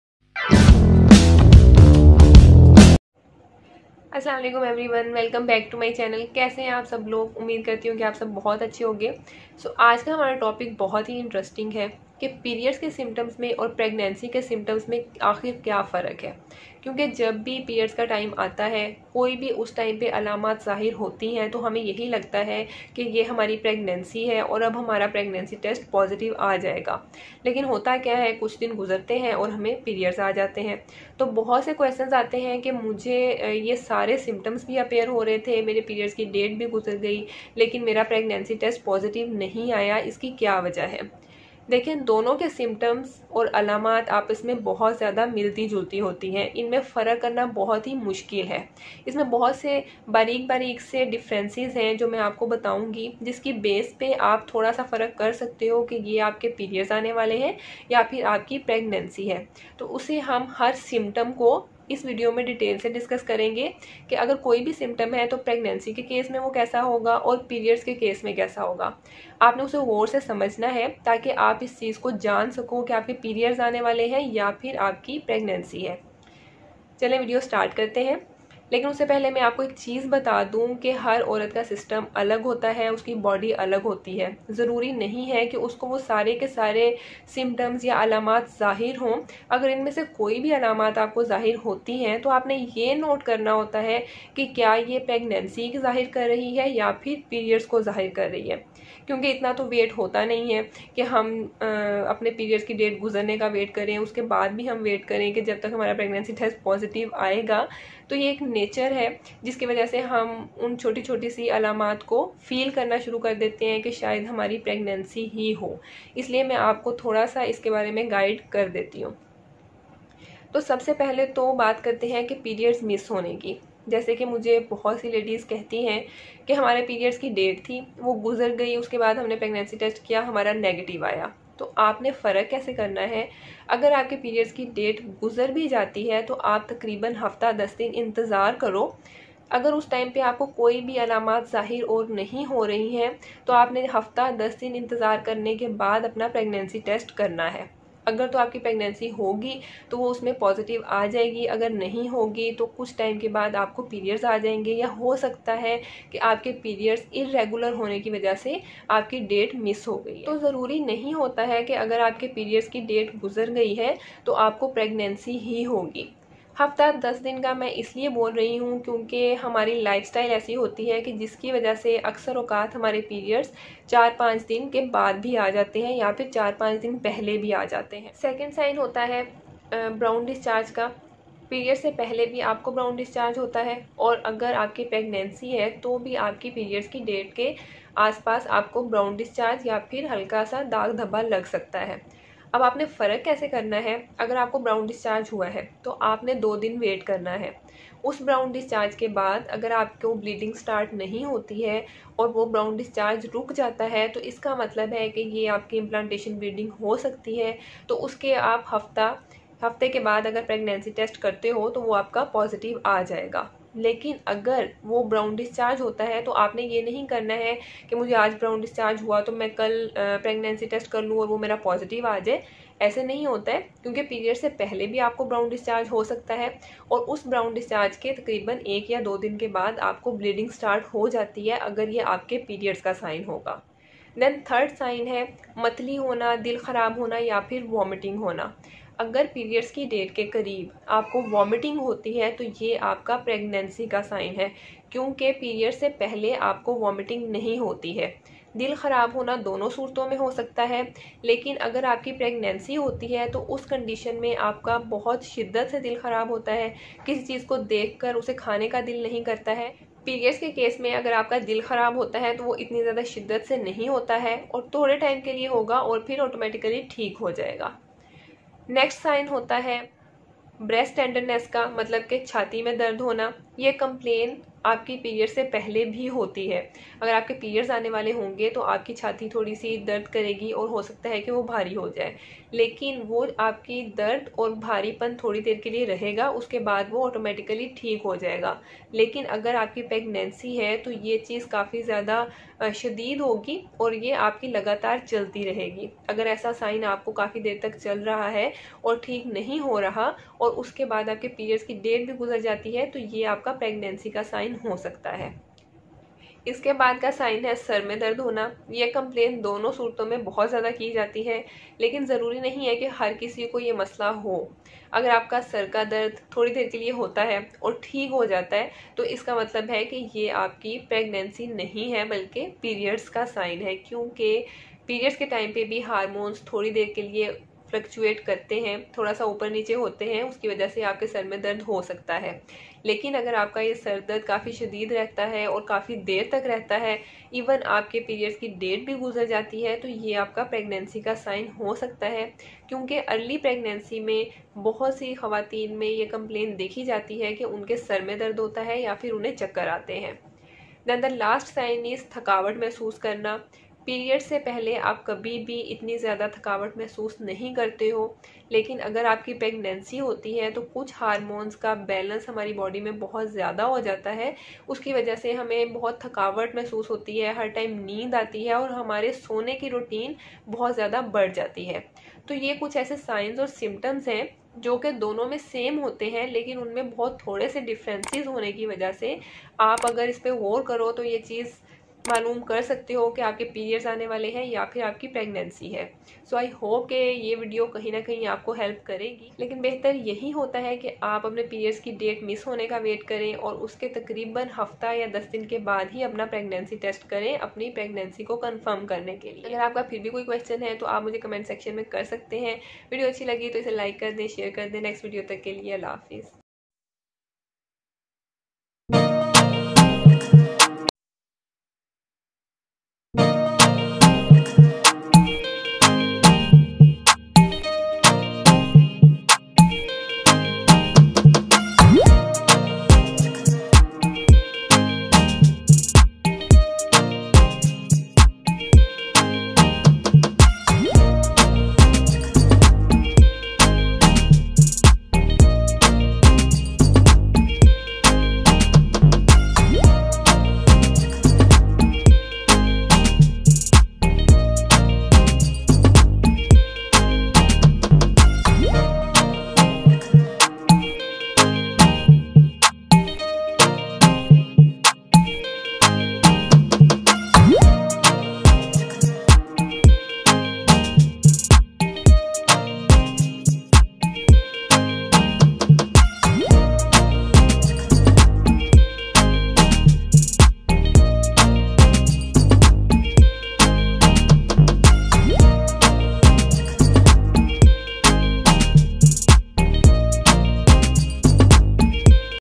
Period symptoms vs Pregnancy symptoms | Early Pregnancy Symptoms | PMS | Mommy Expertise
Early pregnancy symptoms. Period Symptoms. Period symptoms vs Pregnancy symptoms

Difference between PMS and Early Pregnancy Symptoms

PMS :
1- Missed periods
Due to lifestyle and irregular periods.

2- Spotting or brown discharge
Old blood leaves body before periods.

3-Nausea But Not vomiting

4- Breast tenderness
For a small period of time

5- Headache
Little bit and go away after sometime.

6-Tiredness
Not too much.

Pregnancy symptoms :
1- Missed period
Do pregnancy test after a week of missed period to confirm pregnancy.

2-Spotting
Implantation bleeding

3-Nausea and Vomiting

4-Breast tenderness
Pain and heaviness

5-Headache
For longer period of time and due to hormonal changes.

6-Tiredness
Sleepiness and too much laziness

Mommy Expertise